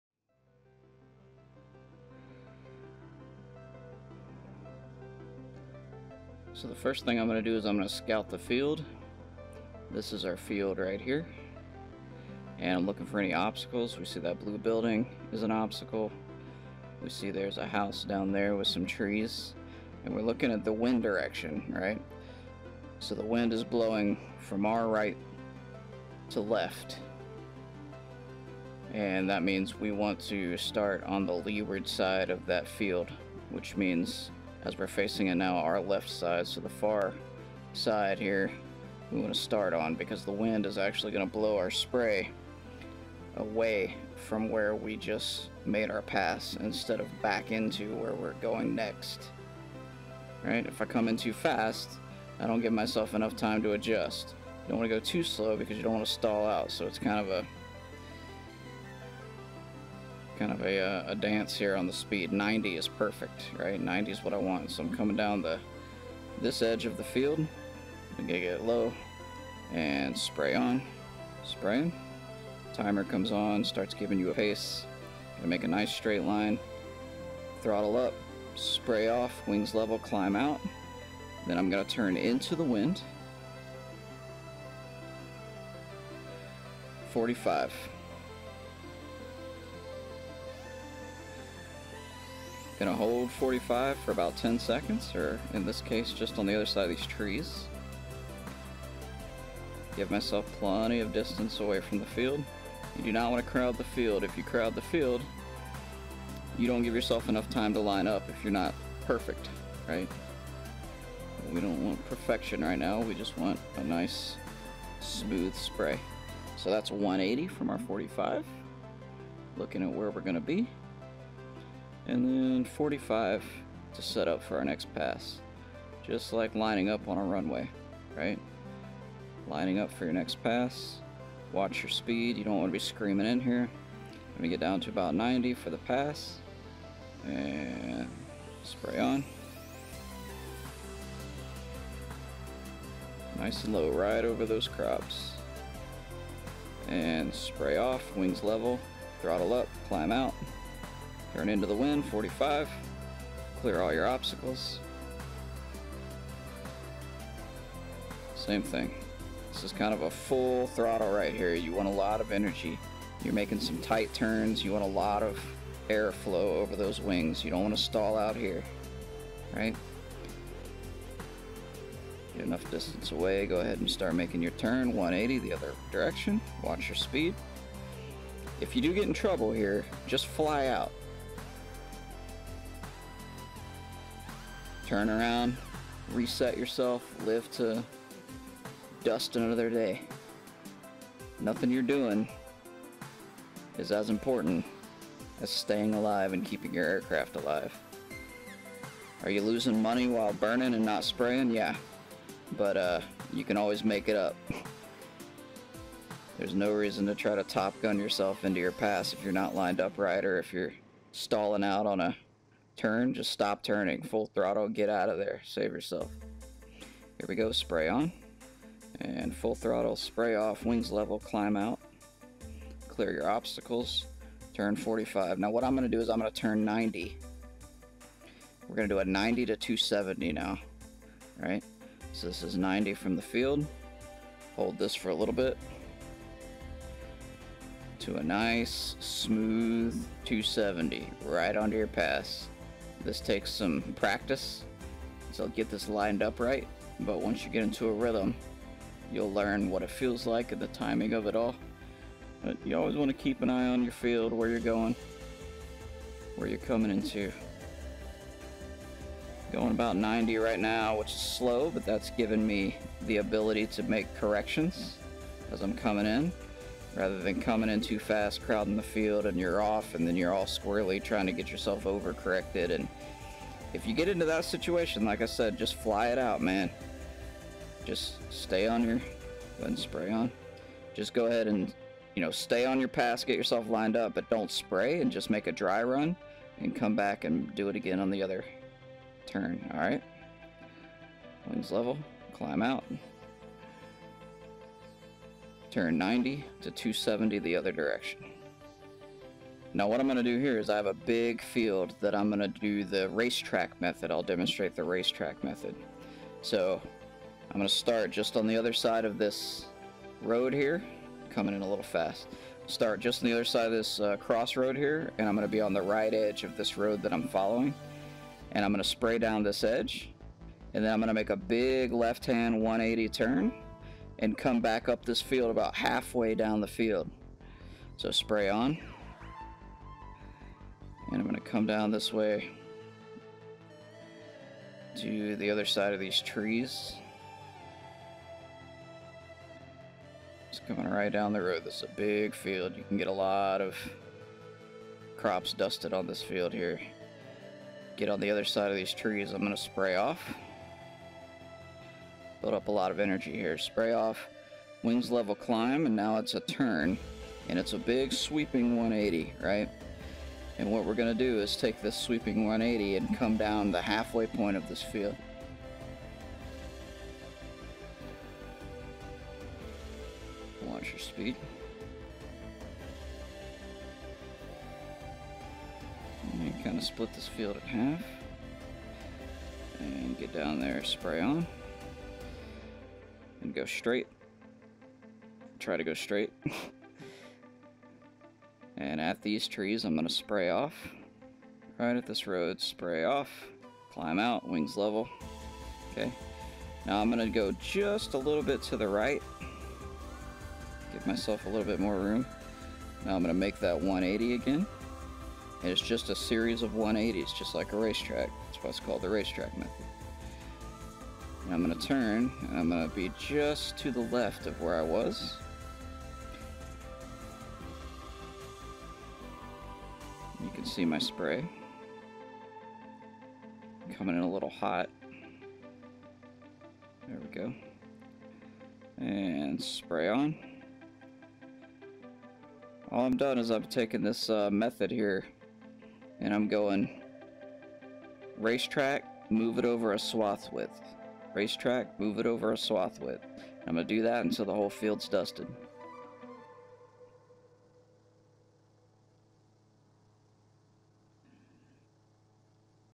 AirSpray Crop Dusting Turns Tutorial: Basic Fixed Wing
Wherein we look at the 3 basic fixed wing Agricultural Crop Dusting turns and get familiar with their basic execution.